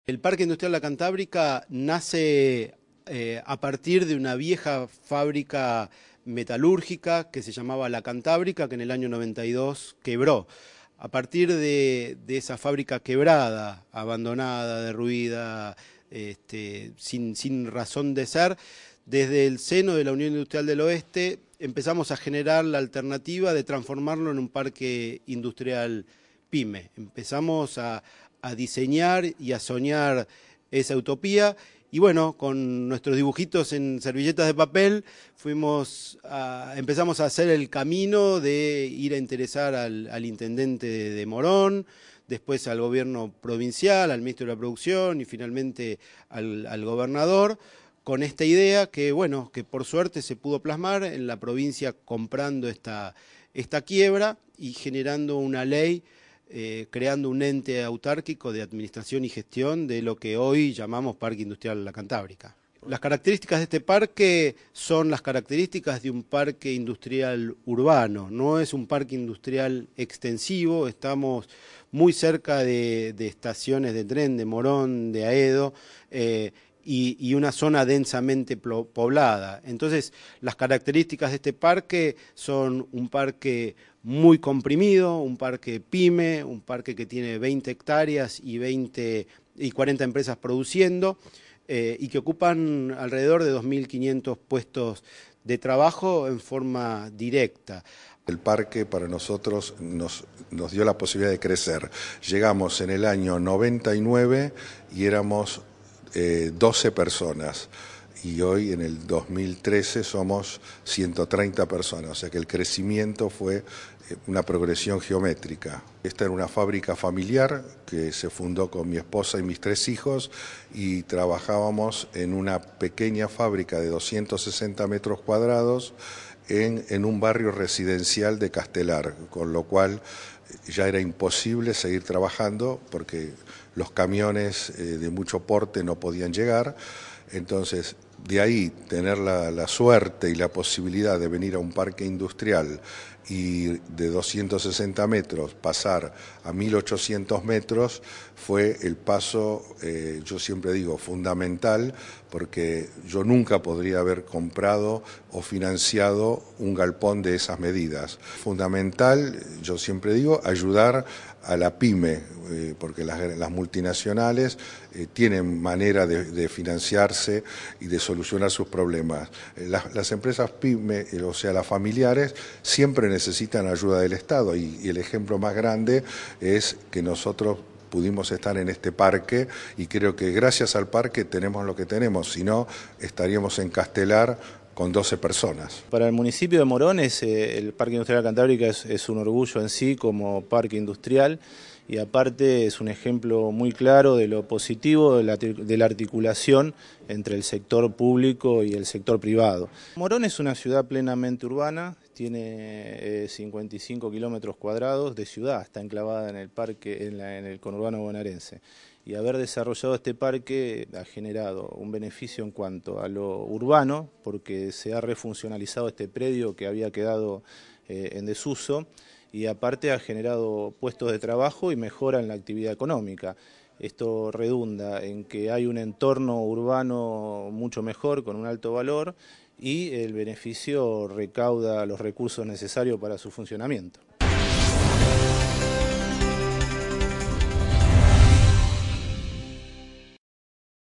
Ex fábrica La Cantábrica alberga hoy 40 empresas y más de 2.500 empleados
Las 20 hectáreas donde se levantaba la fábrica La Cantábrica en los años 80, luego de la quiebra y su total vaciamiento en los 90, albergan hoy a más de 2500 empleados que volvieron al sector productivo y se desempeñan en las más de 40 empresas que se instalaron, transformando el lugar en el flamante Parque Industrial de Morón.

Edgardo Gámbaro, a cargo del funcionamiento del lugar y uno de los primeros empresarios en instalar su firma en el complejo, contó a Télam que "hubo que levantarlo de la nada. Un grupo de industriales nos fuimos juntando y de a poco, con el apoyo del Estado, pudimos convertir esto en un polo productivo".

"La Cantábrica era un ícono, una industria metalúrgica con más de 8.000 empleados. Y luego de su destrucción total, con los años, fuimos levantándola un grupo de industriales. Empezamos con cinco empresas. Hoy somos más de 40, dedicadas a diferentes rubros, y todas tenemos características pyme", agregó.

El parque "La Cantábrica" cuenta con plantas de tratamiento de efluentes, y allí se producen cubiertos descartables, juguetes, precintos de seguridad, máquinas blisteras, metalúrgicas, grupos electrógenos, productos químicos, componentes telefónicos y eléctricos, calzado, telas, ventiladores industriales, matricería e inyección, impresiones gráficas, entre otros artículos y servicios.

Gámbaro, quien además es presidente de la Unión Industrial del Oeste y de la Federación Argentina de Parques Industriales, señaló "la estructura industrial periférica que se potencia con este polo productivo" y puntualizó acciones que permitieron una optimización de zonas aledañas, como por ejemplo, la construcción de cloacas para el parque que beneficiaron a varias manzanas del barrio La Cantábrica, y la pavimentación de las calles de acceso al polo fabril.

Por otra parte, el predio fue distinguido como el Primer Parque industrial con Responsabilidad Social Empresaria (RSE) por parte del Ministerio de Desarrollo Social.

En este sentido, Gámbaro contó que trabajan cotidianamente con "alumnos de escuelas técnicas que han llegado a desarrollar productos tan interesantes como por ejemplo un casco inteligente para motos que suena cuando el motociclista no lo lleva puesto, u otro caso de una silla de ruedas que por mecanismos específicos permite poner en posición vertical a la persona que está sentada y necesita determinados movimientos motrices según su patología".

Carlos Bustamante, titular de la firma Good Food SA., contó que se dedican a la fabricación y comercialización de porciones individuales y hacen trabajos para terceros de alimentos de cualquier tipo. Hoy tienen 130 personas trabajando pero cuando comenzó a desarrollarse en el parque, en el año 1999, eran 12 empleados.

"El crecimiento fue en progresión geométrica. Eramos una empresa familiar que trabajaba en una pequeña fábrica de 200 metros cuadrados en una zona residencial de Castelar y por el traslado de los camiones, se nos hacía imposible seguir ahí. Sin la posibilidad de pasar a estos 1.800 metros que tenemos hoy en el parque, hubiera sido imposible despegar de esta manera", dijo.

Agregó que el galpón donde producen era "el vestuario de los empleados de La ex Cantábrica, y pensar que hoy producimos, por ejemplo, condimentos para una firma de comidas rápidas líder en el mundo, y los abastecemos en sus más de 220 comercios".

Bustamante destacó que "excepto el 2001 siempre crecimos. Acabamos de comprar un galpón nuevo de 1.200 metros donde hicimos una loza y lo extendimos a 2.000 metros. Esto fue hace dos años, y ya nos quedó chico nuevamente".

El empresario puntualizó que "las políticas de reindustrialización nos favorecieron. Recibimos préstamos de FONAPYME, de la SEPYME, todo lo que se nos ayudó desde el Estado, lo supimos aprovechar".

Agregó que su firma, en los últimos 10 años, "creció 50 por ciento en gente, en ventas y ediliciamente".

José Brito trabaja en el restaurante del parque desde el 2001 y contó que "los clientes tienen un sector para trabajadores y un sector vip donde van los empresarios. El sector vip tiene capacidad para 60 empresarios y el de trabajadores para unos 200 aproximadamente. El menú es el mismo en los dos lugares aunque los costos en el vip son un poco más caros".

Brito manifestó orgulloso que en el parque "tenemos de todo, desde productores de dulce de leche hasta empresas gráficas" y dijo que "vemos que hay mucho movimiento sobre todo los últimos años. Lo demuestra que antes éramos 3 mozos y ahora ya somos 12".

El restaurante funciona solamente al mediodía y algunos sábados se cierra para eventos vinculados a presentaciones de productos según el requerimiento de alguna de las firmas que integran el complejo.